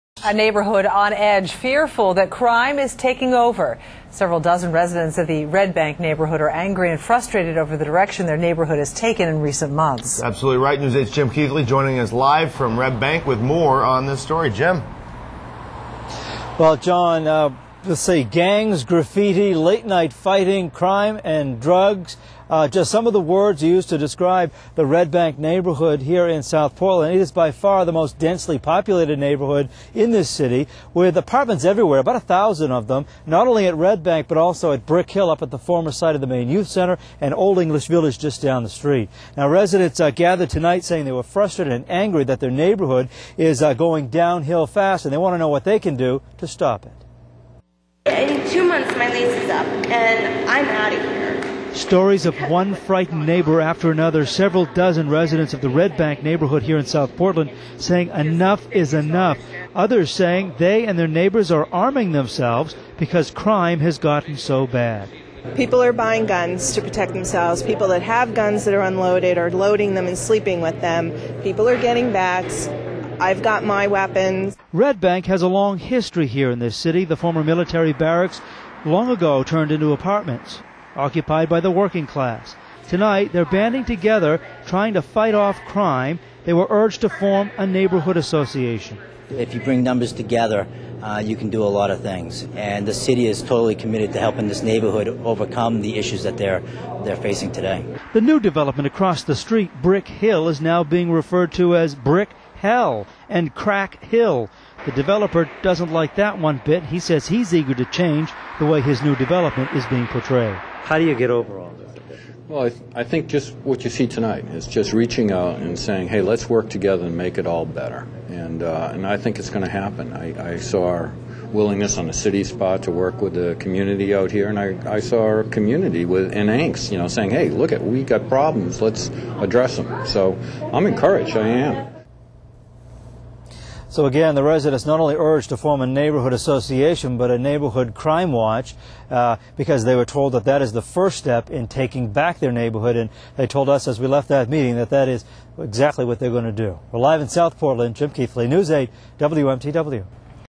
Rising Crime Worries Neighborhood
Residents of a South Portland neighborhood say they are concerned about rising crime, and they wnat something done. News 8's Jim Keithley reports.